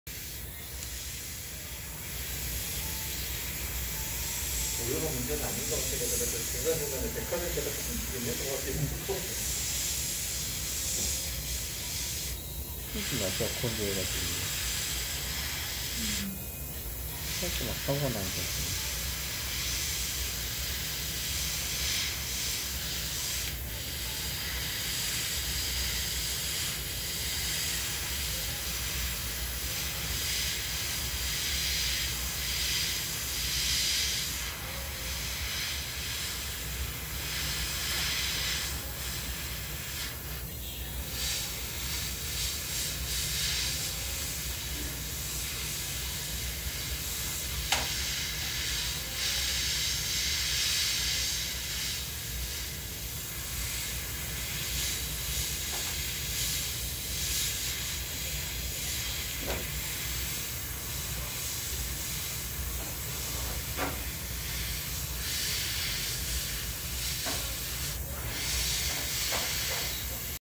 튜닝타임즈 강좌 명동 정화예술대학교 평생교육원 OOO님의 넨도 오비츠 커스텀 앙상블스타즈 Tomoe Hiyori 토모에 히요리 에어브러시 색칠 작업
명동 정화예술대학교 평생교육원 넨도로이드 오비츠 커스텀 수업 수강생 OOO님이 서페이서를 칠해줬던 머리 부품에 흰색을 덮어서 밑색작업을 해주셨고요, 그 위에 짙은 색상으로 녹색을 먼저 칠해가며 에어브러시로 그라데이션 색칠을 해주시고 있습니다^^